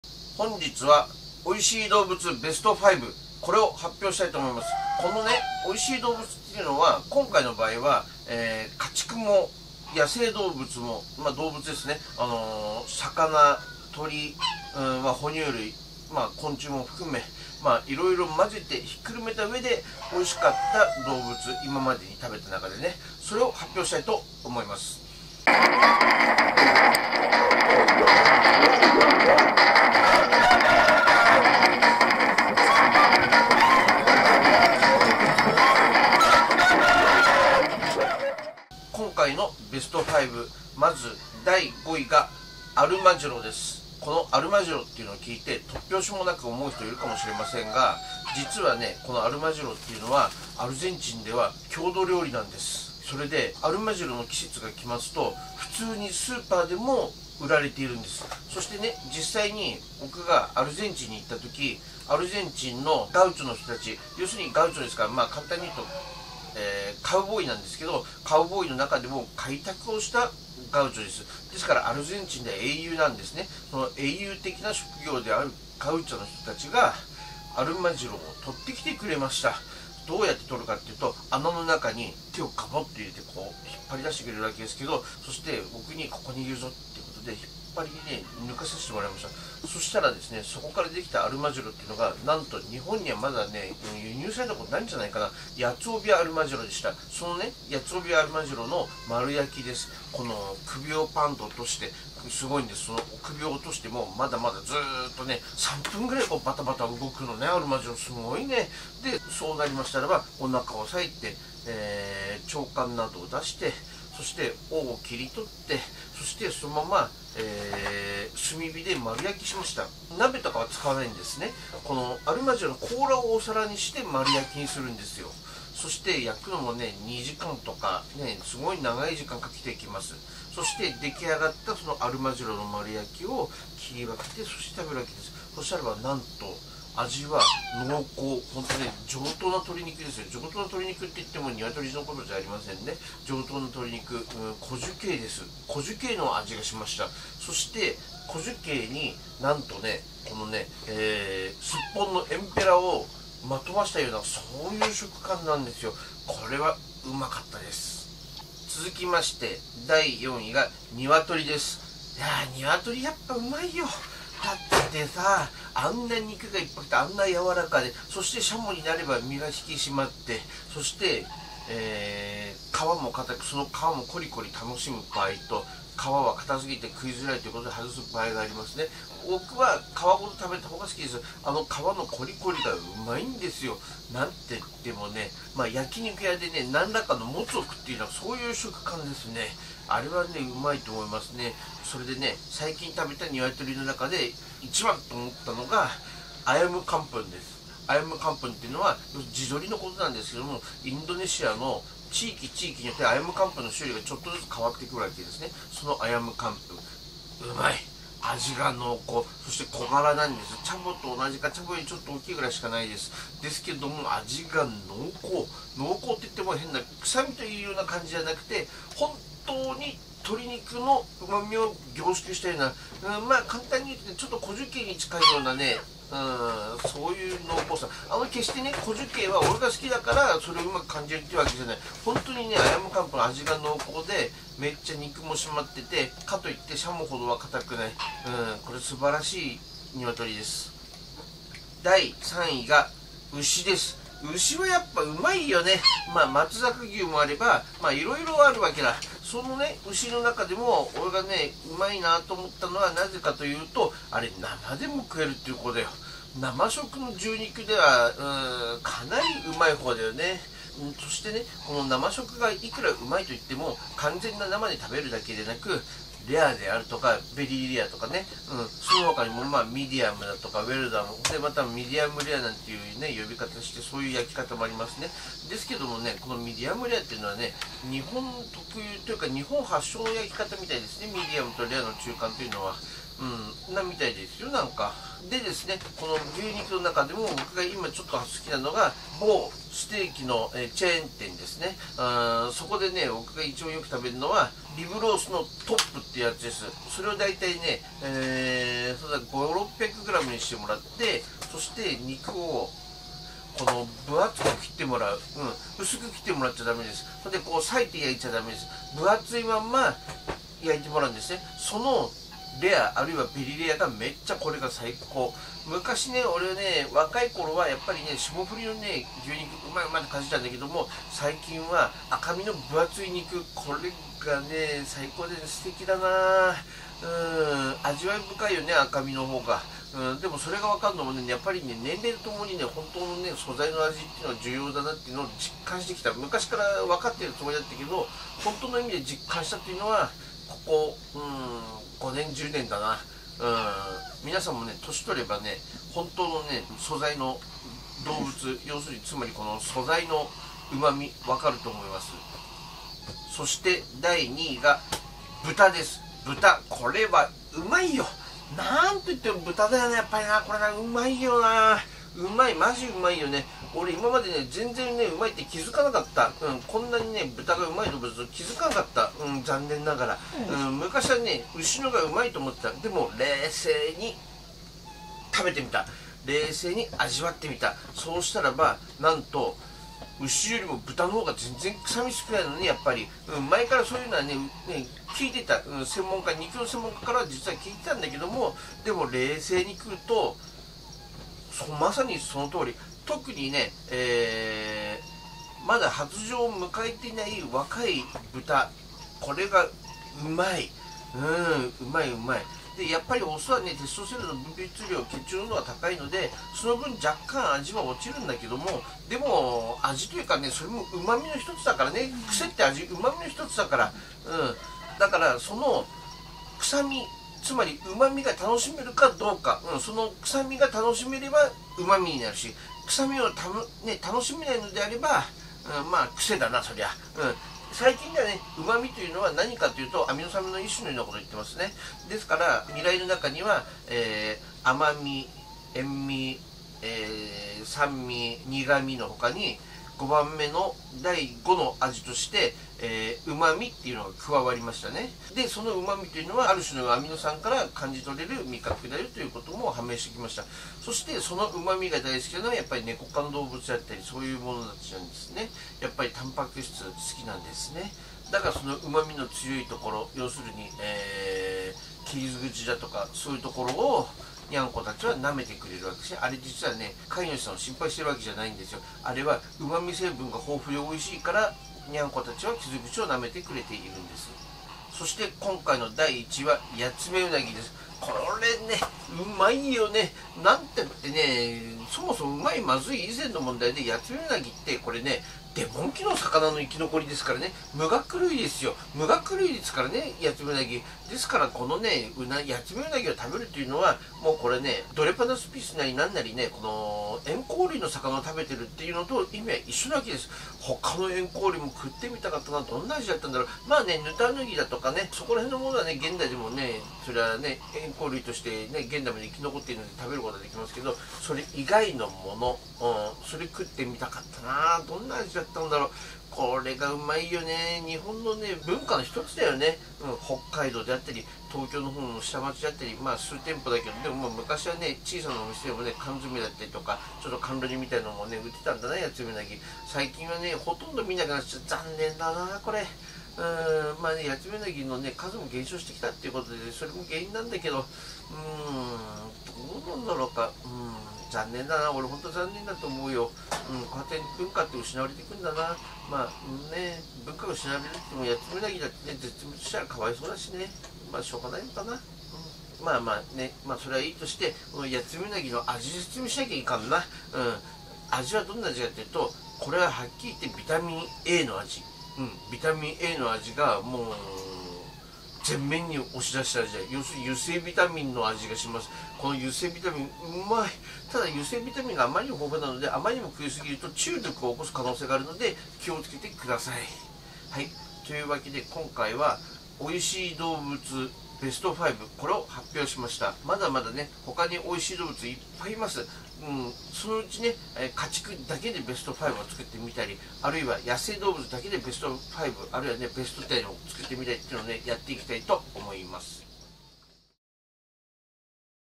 美味しい動物ベスト5！
パンク町田が世界各地で食べた様々な食材の中でおいしかった動物ベスト5を発表します！
これまでに家畜から野生動物、昆虫まであらゆる動物を食べてきた食通パンク町田を唸らせた動物とは？

00:00 美味しい動物ベスト5！
00:38 第5位！
02:38 第4位！
04:01 第3位！
05:53 第2位！
07:55 第1位！

☆☆☆パンク町田のアニマルTV 関連動画

☆☆☆SNS

パンク町田の生物行動進化研究センターX

☆☆☆関連サイト